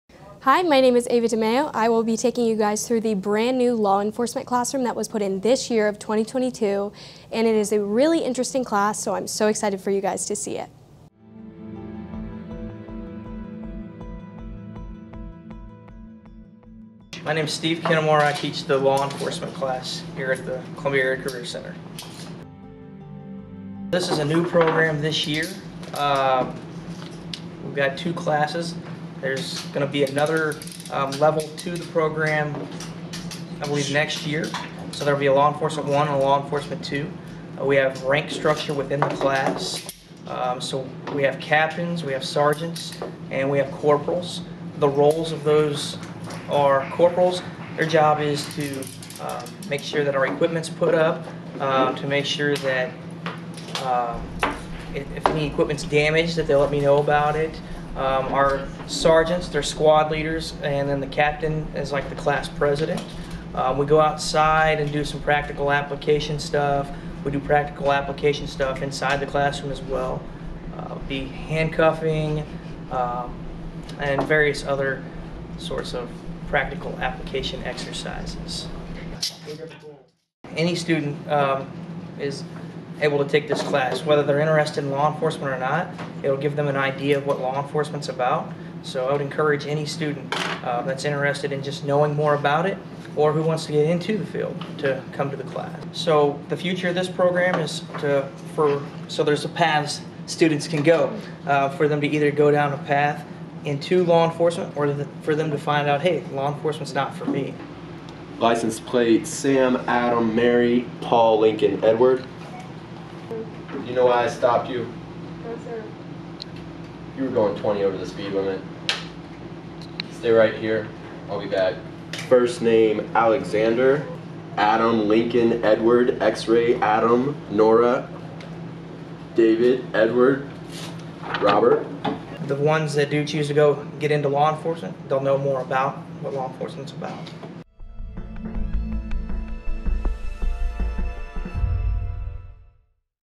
Law Enforcement 2022-23
Join Ava DiMeo as she explores the new Law Enforcement program at Columbia Public Schools' Columbia Area Career Center.